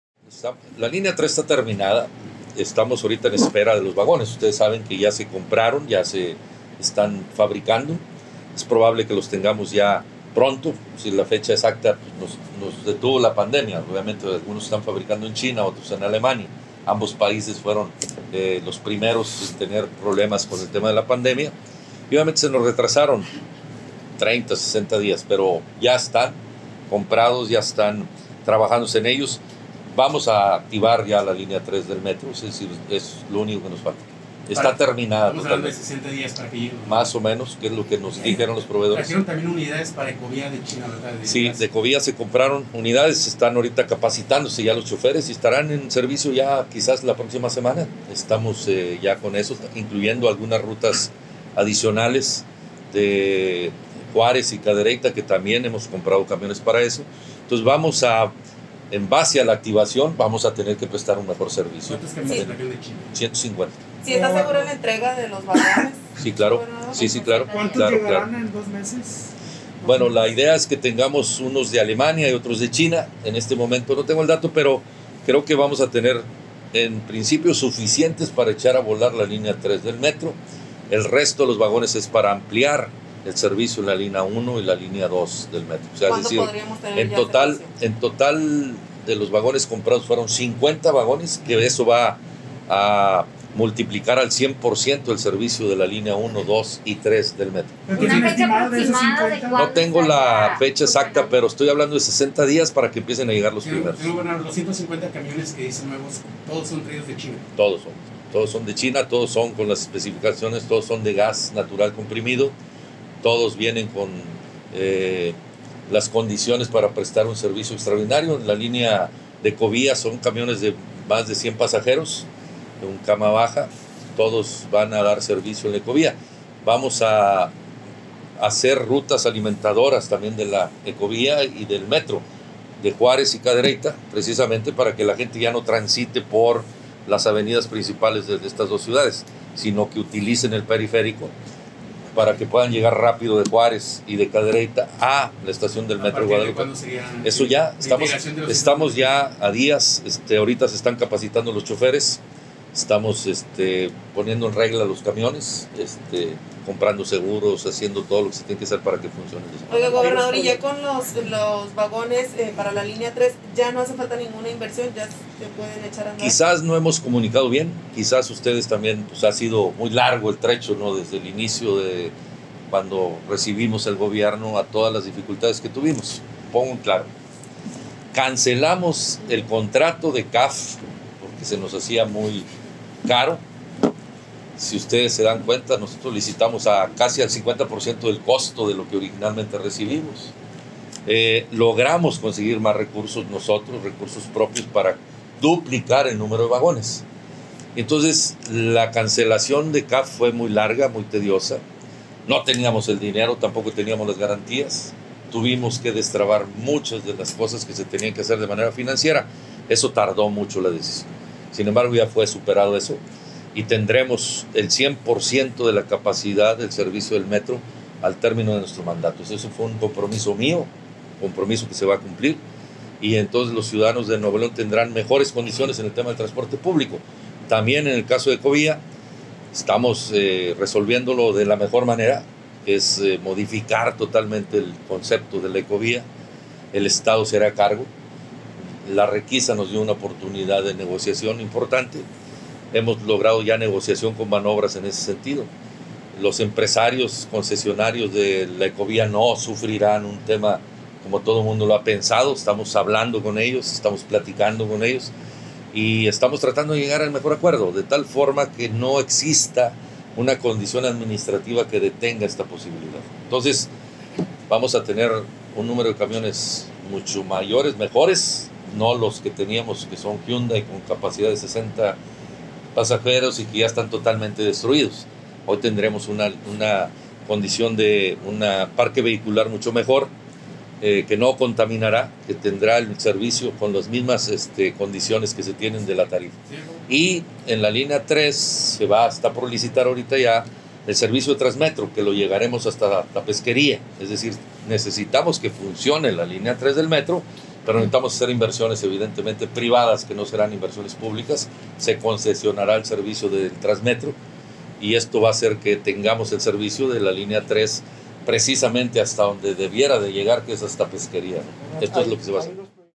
Resolviendo dudas sobre la Línea 3 del metro
Sé que muchos tienen dudas sobre por qué aún no está en funcionamiento la línea 3 de Metrorrey. Por este motivo les comparto un audio en el cual aclaro este tema e informo acerca de las nuevas unidades de Ecovía y rutas alimentadoras que mejorarán la movilidad en el estado.